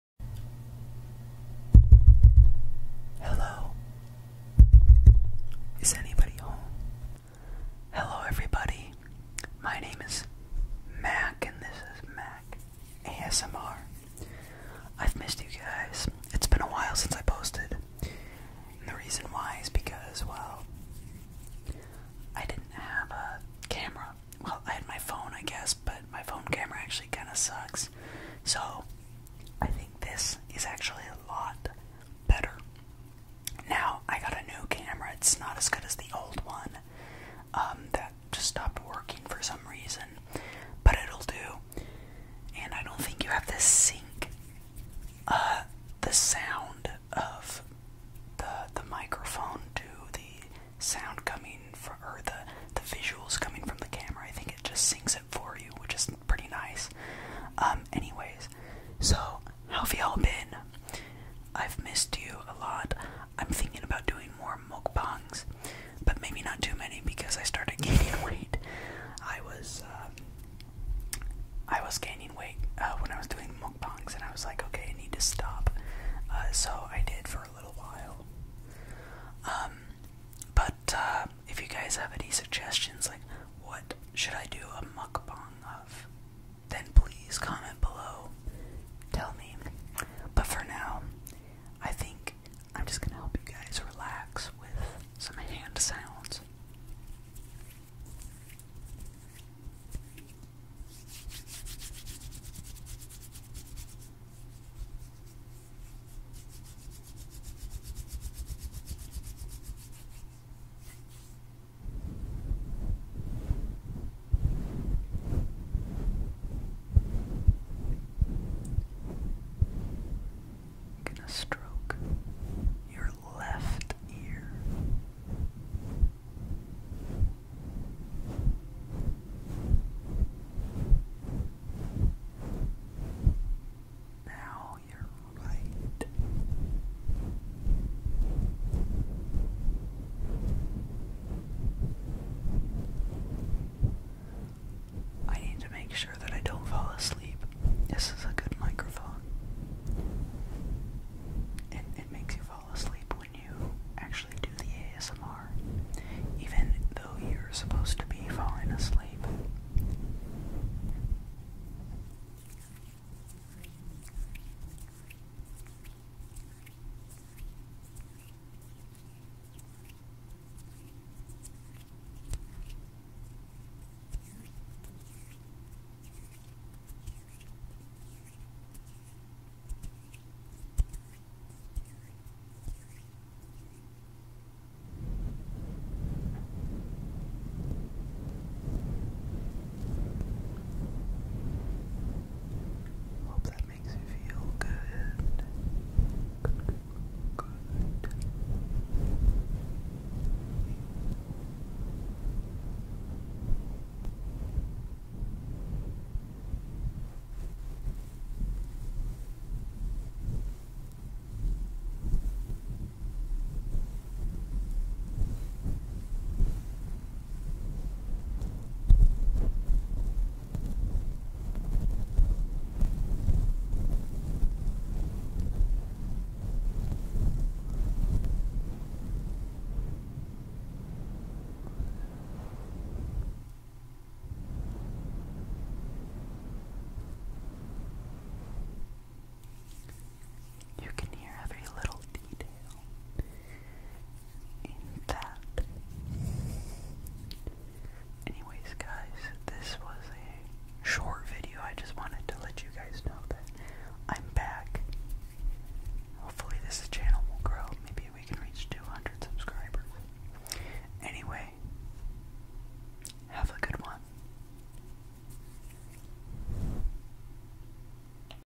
New Vid! New Camera! ASMR
Hey guys, it's been a while since I've posted. Yeah, my camera is not as good, but it gets the job done, and I don't have to sync the audio with the visual!
Bye!